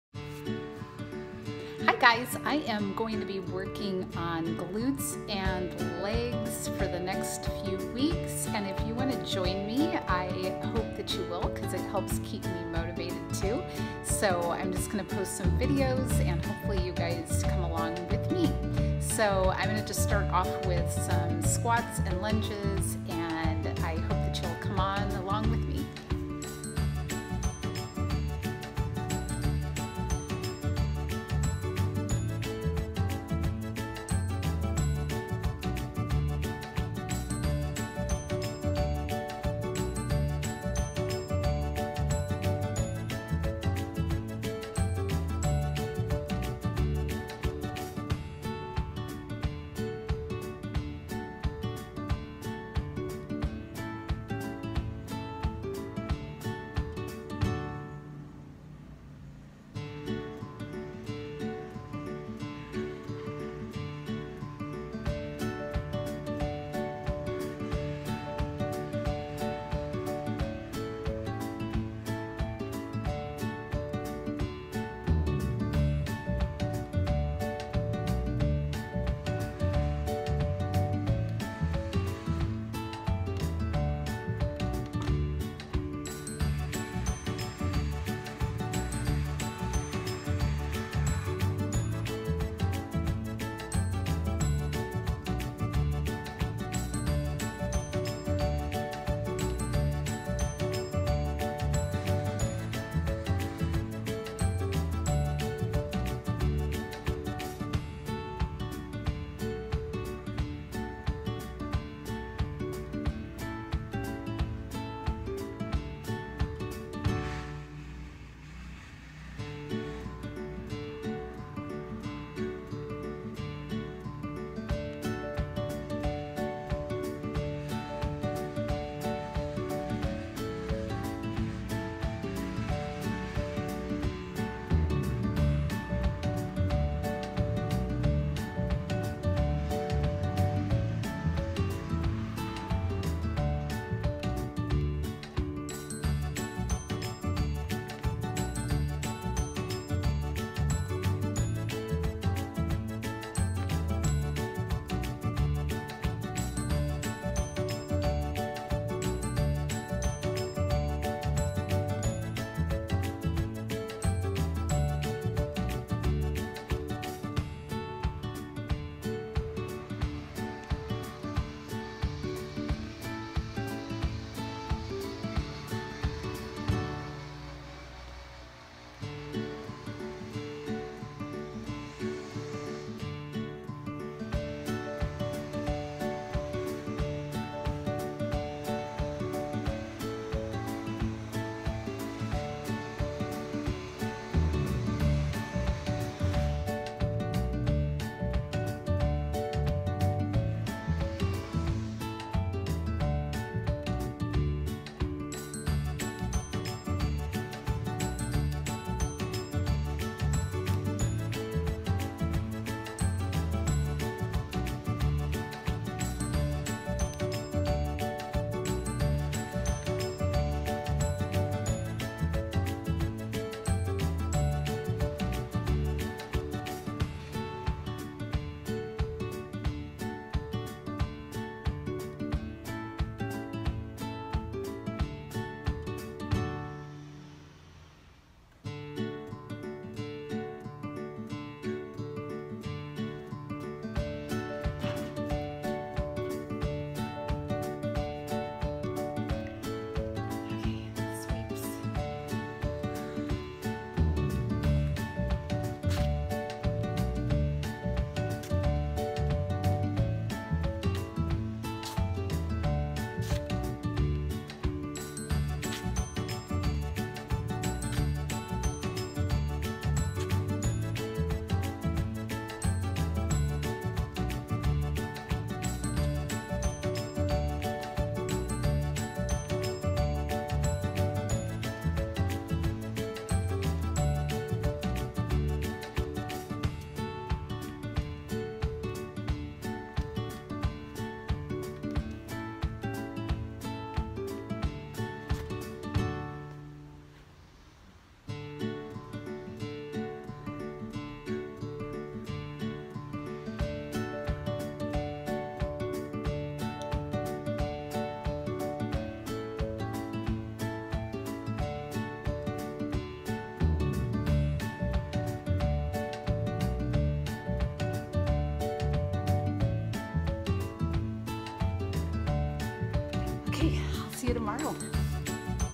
Legs & Glutes
Join me! I’m working on them for the next few weeks and it’s always more fun together!
Let me know if you do them too!
Today:
✅50 squats (I do them slightly modified because of my belly surgeries)
✅20 alternating lunges
✅10 leg sweeps

Let’s go!